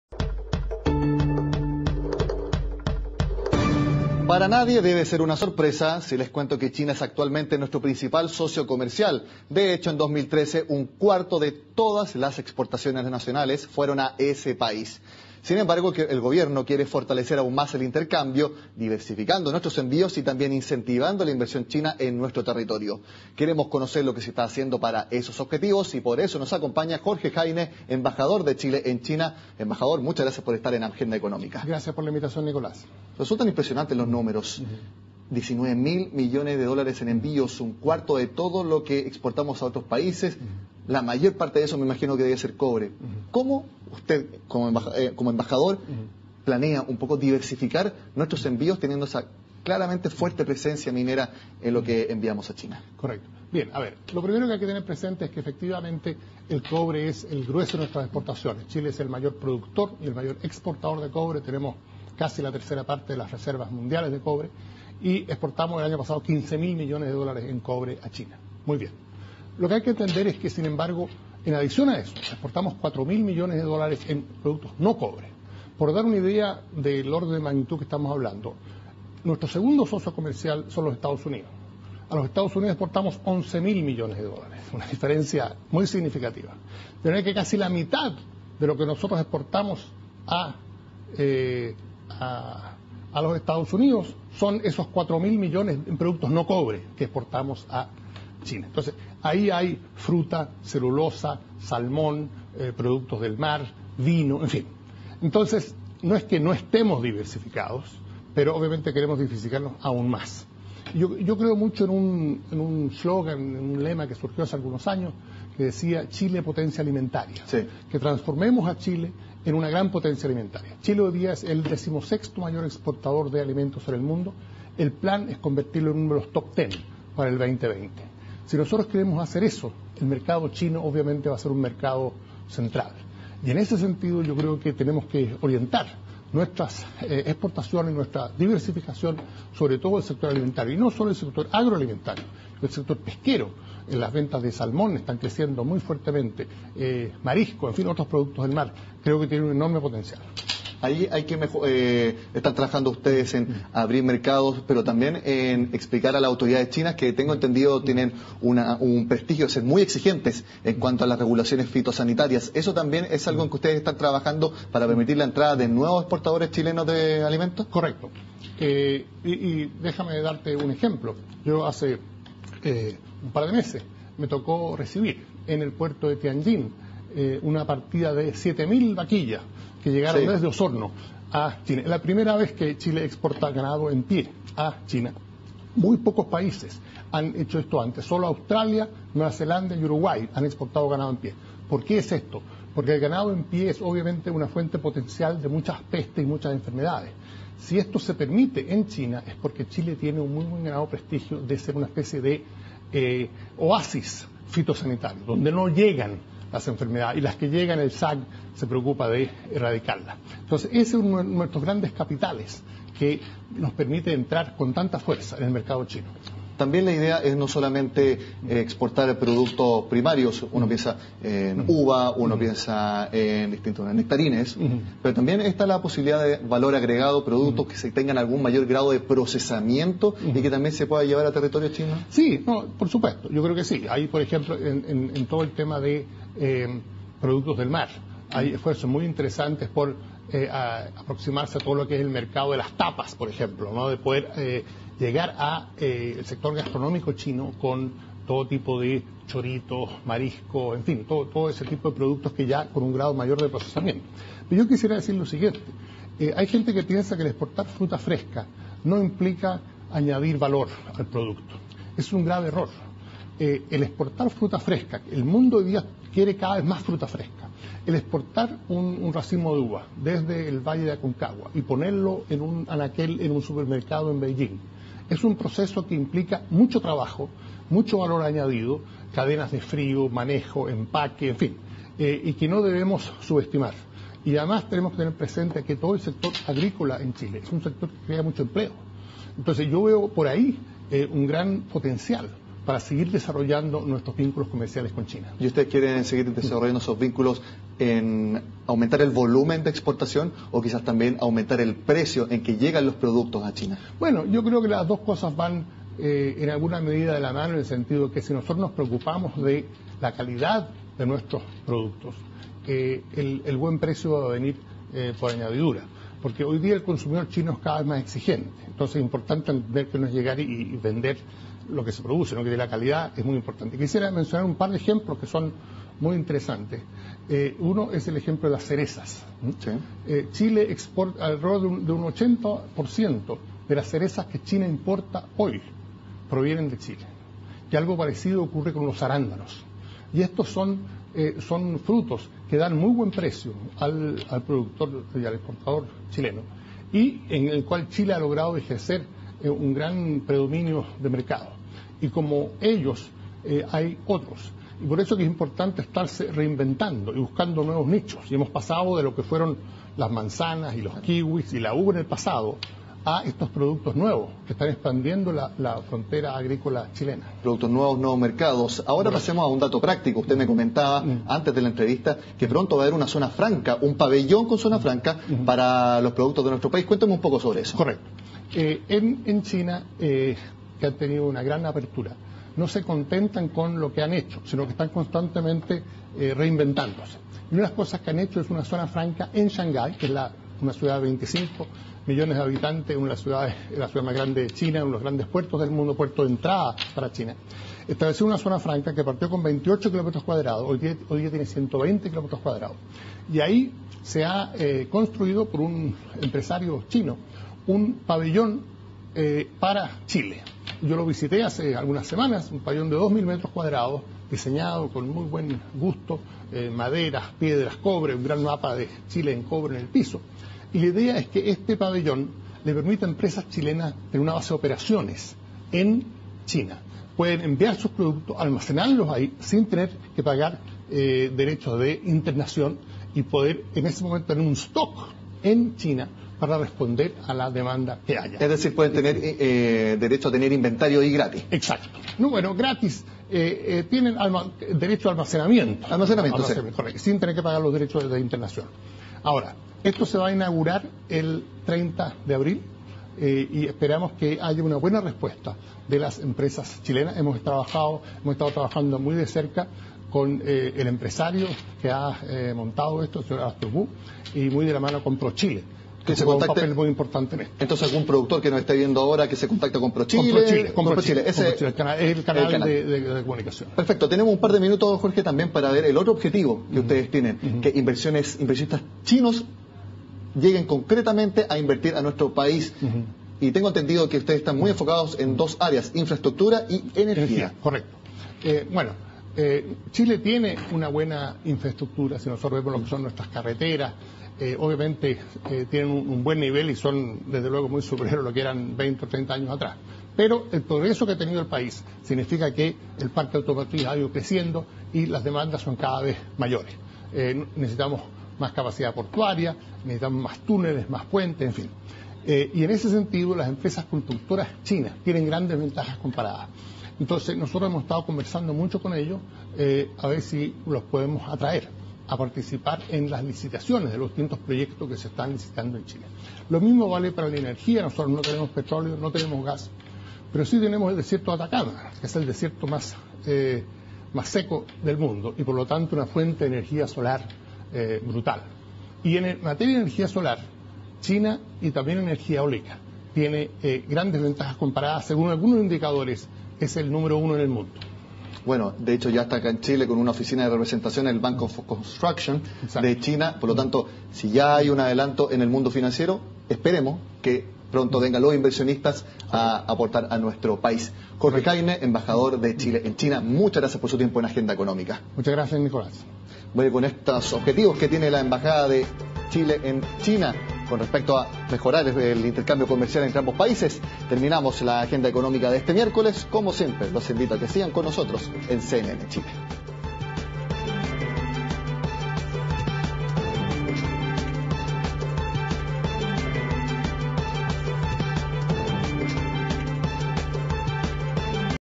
Los objetivos para fortalecer el comercio entre Chile y China
El cobre es el grueso de las exportaciones hacia el país asiático, por lo que los esfuerzos están puestos en diversificar más la gama de productos.